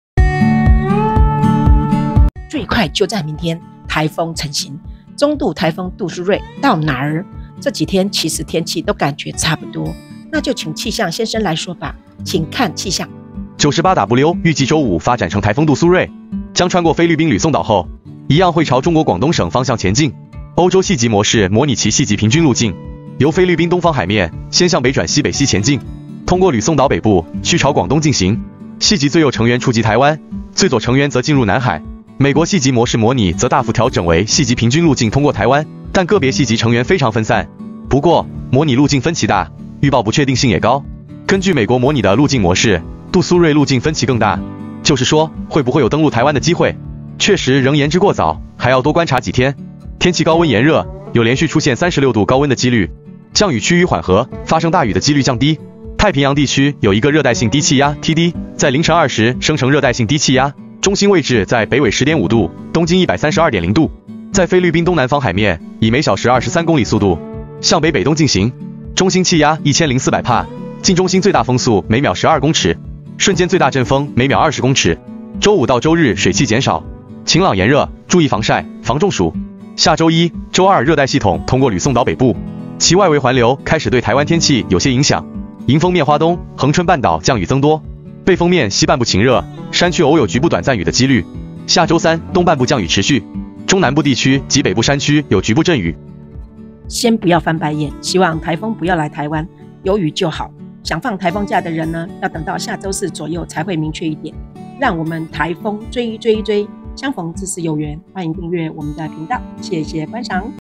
實時路徑 中颱杜蘇芮到哪兒 最快就在明天颱風成形
會不會有登陸台灣的機會，確實仍言之過早，還要多觀察幾天。天氣高溫炎熱，有連續出現36度高溫的機率，降雨趨於緩和，發生大雨的機率降低，太平洋地區有 1 個熱帶性低氣壓(T.D.)在凌晨2時生成熱帶性低氣壓，中心位置在北緯 10.5 度，東經 132.0 度，在菲律賓東南方海面，以每小時23公里速度，向北北東進行。中心氣壓1004百帕，近中心最大風速每秒12公尺，瞬間最大陣風每秒 20 公尺。週五到周日水氣減少，晴朗炎熱，注意防曬、防中暑；下周一、周二熱帶系統通過呂宋島北部，其外圍環流開始對台灣天氣有些影響；迎風面花東、恆春半島降雨增多，背風面西半部晴熱，山區偶有局部短暫雨的機率。下周三東半部降雨持續，中南部地區及北部山區有局部陣雨。